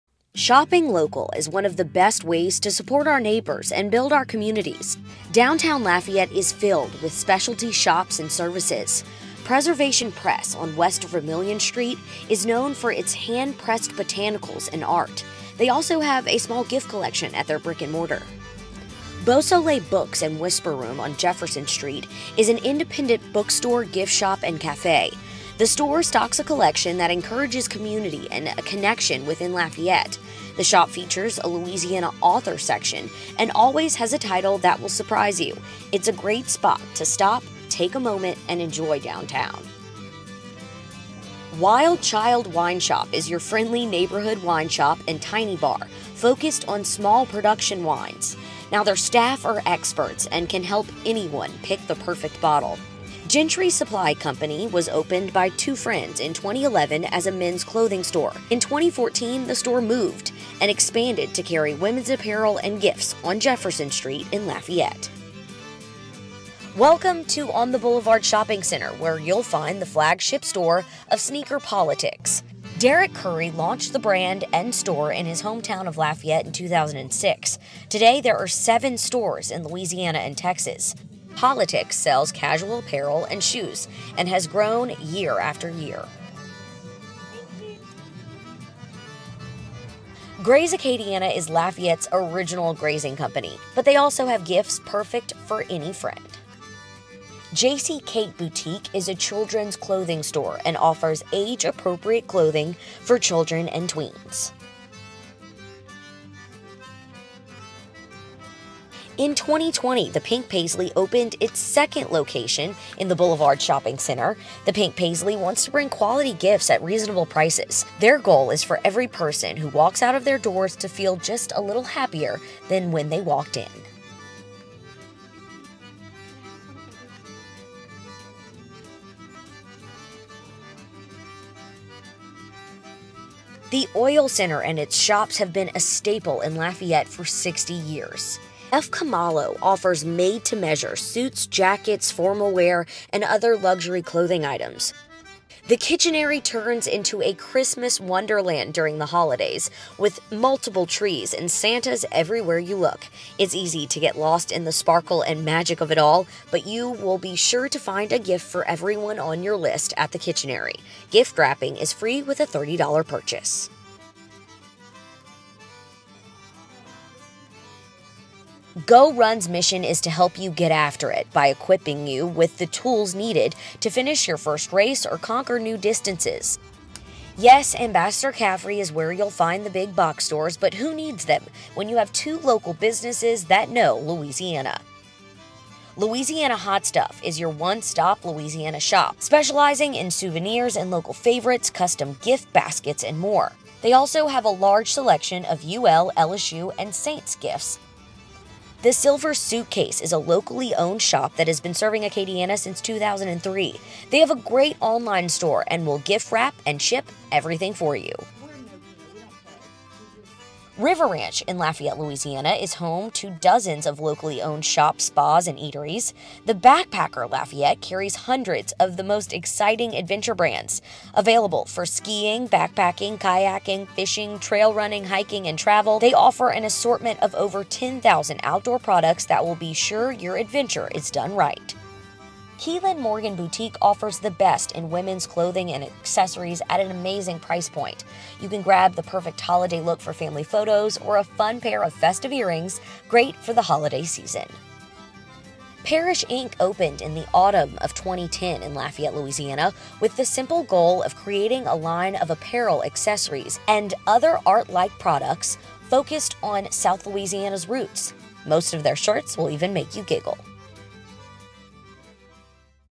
Small Business Saturday in Lafayette, LA
Shopping local is one of the best ways to support our neighbors and build our communities! Small business owners give everything to the places that make Lafayette, LA's shopping experience unique. Could you imagine Lafayette without the charm of our local gift shops, art galleries, boutiques, and craft shops? It’s hard to picture life without our favorite local stores filled with true customer service and people who care! That’s why it’s important to shop locally. Here are just a few locally-own